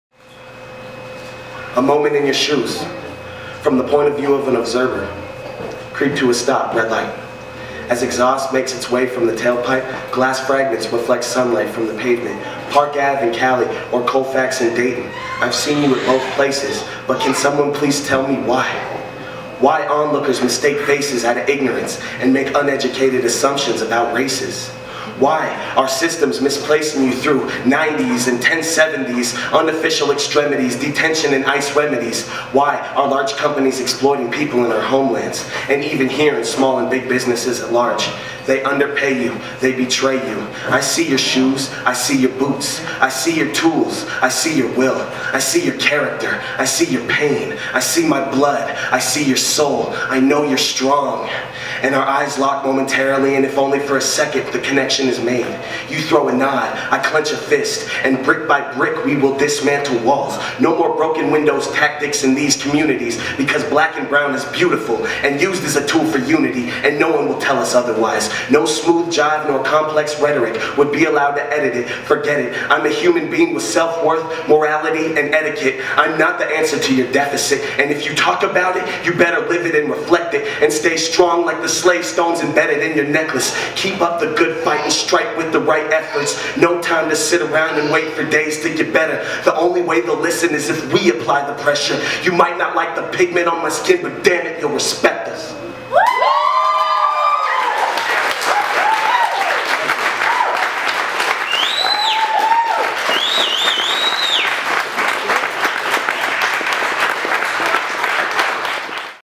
Alex Landau Poem from an Evening of Storytelling
A short clip from the Evening with Storytelling with the Romero Theater Troupe on 11/15/13. Alex Landau recites a poem he recently wrote.